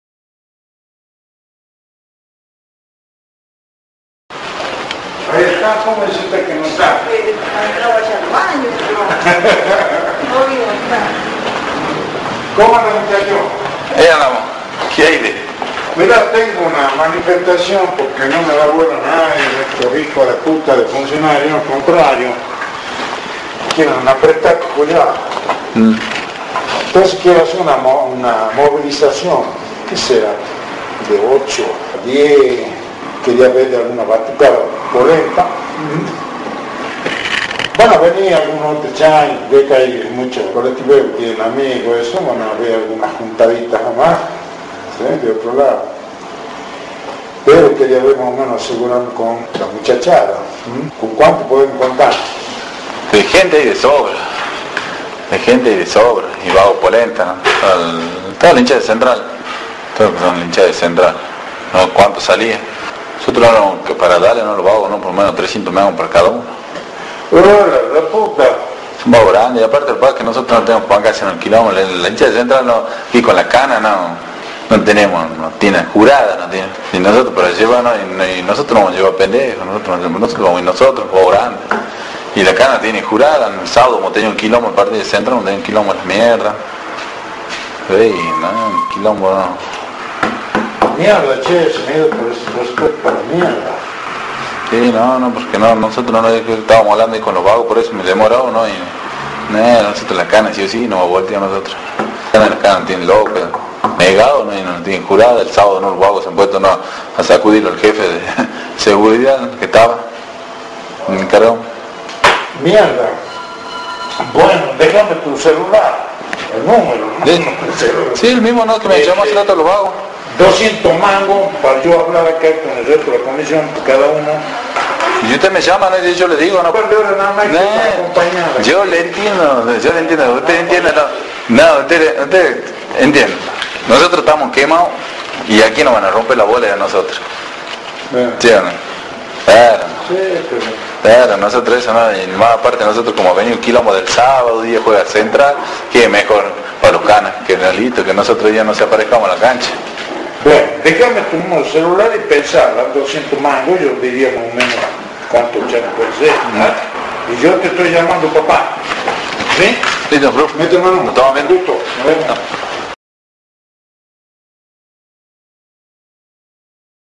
Oscar Cruz quiso contratar barras de Central Norte para una movilización de UTA
9/11/2011 - Cruz organiza una manifestación y pidió el "apoyo" de un grupo de hinchas azabaches